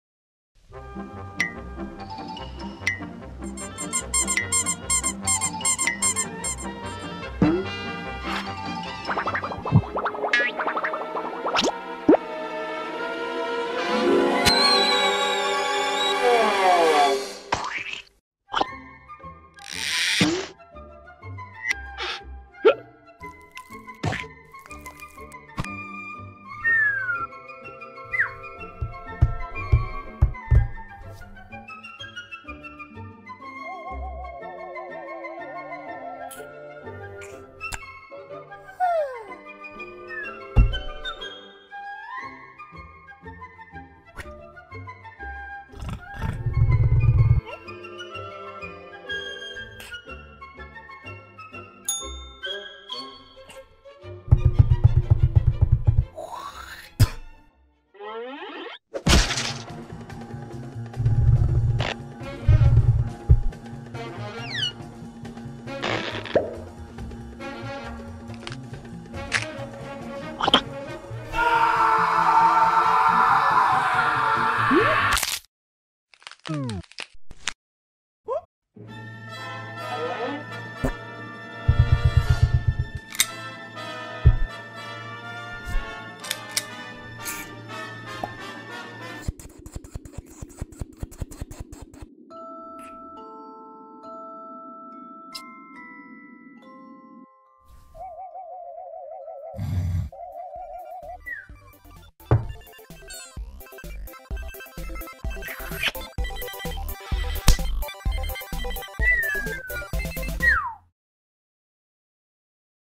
Pinko and the Mouse (2020)
pinko discovers a small creature living inside the walls of her house and decides to become the alpha

another crude animated short i made for class the night before it was due which probably isnt good for me